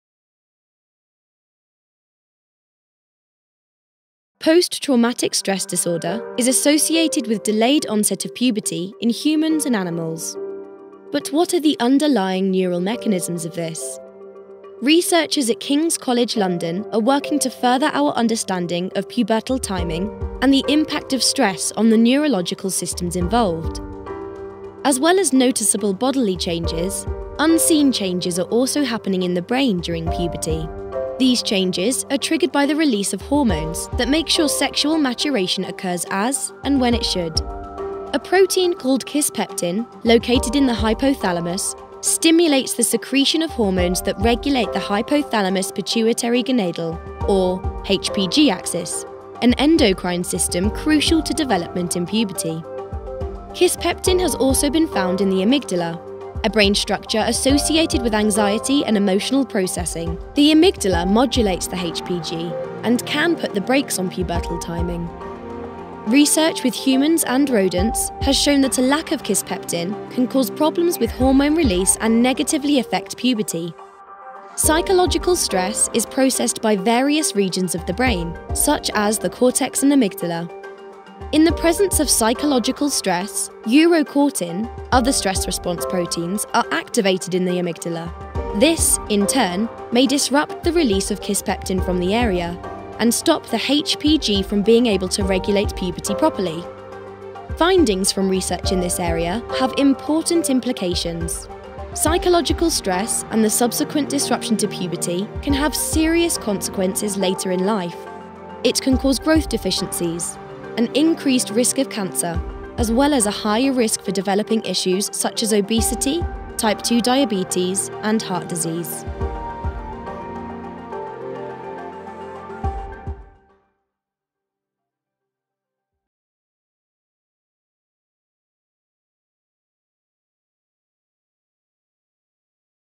Stress and altered pubertal timing: Is the limbic brain the key?
Pubertal onset is controlled by complex neuronal networks in the brain. Remarkable progress has been made in our understanding of the neural regulation of puberty, however the exact timing mechanism of puberty and it’s modulation by stress remains to be fully elucidated. Traumatic stress can alter pubertal timing independent of adiposity. This is concerning as altered pubertal timing can have deleterious effects on the health of an individual later in life.